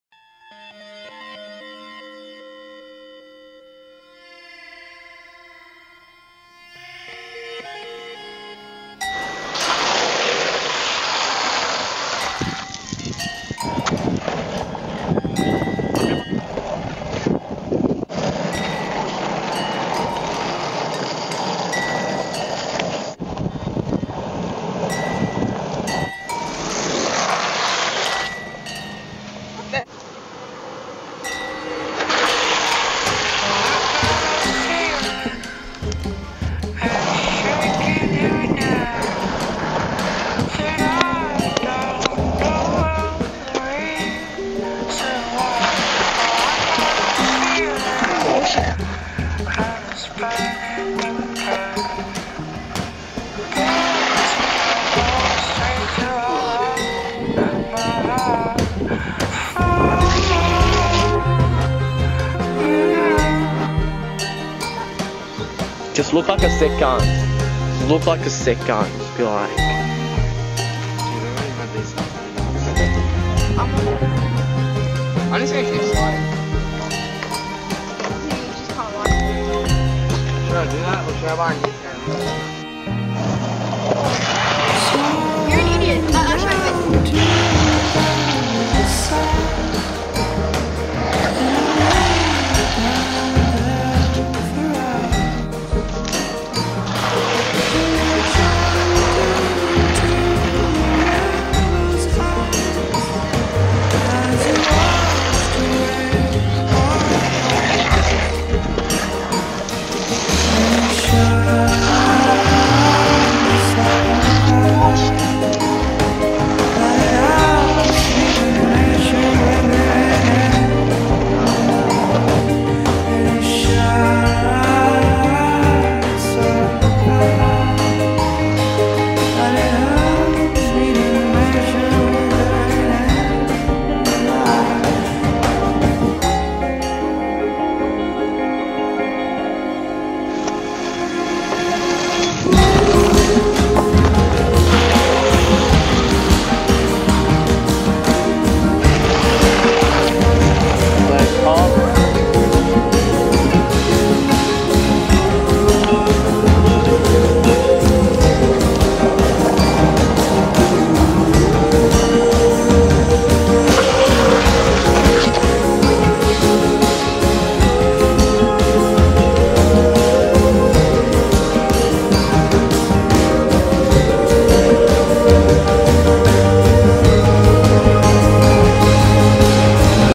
Original Shredding
Kasatafilms Presents

Original Shredding (At North Epping and Other Places)

Riders: - James Ramm
           - Tom Cleary
           - Sam Clare
           - Carl Gai